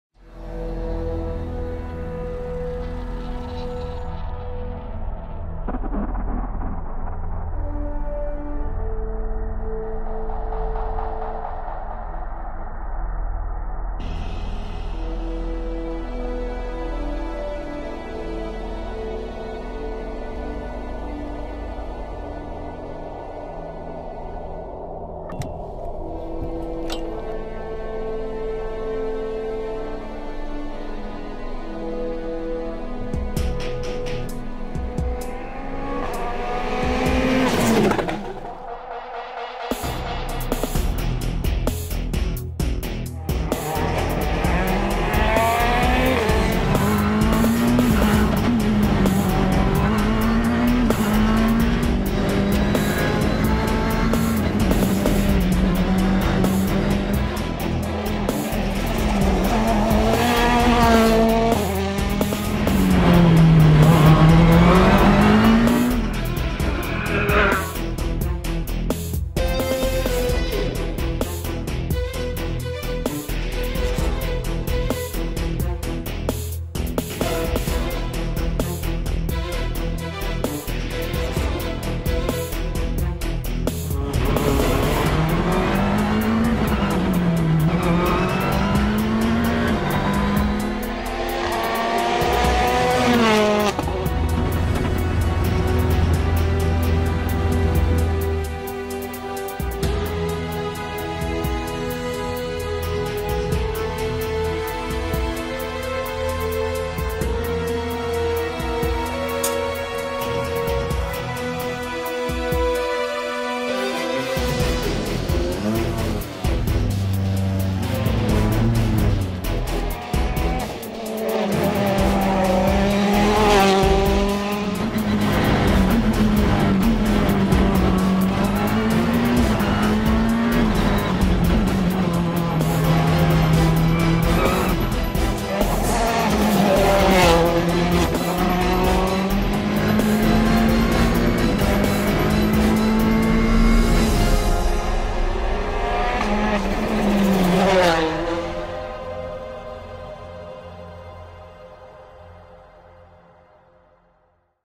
Ostberg testing - 2014 WRC Rallye Monte-Carlo - Best-of-RallyLive.com
Watch Mads Ostberg's tests for the Rallye Monte-Carlo with the Citroën DS3.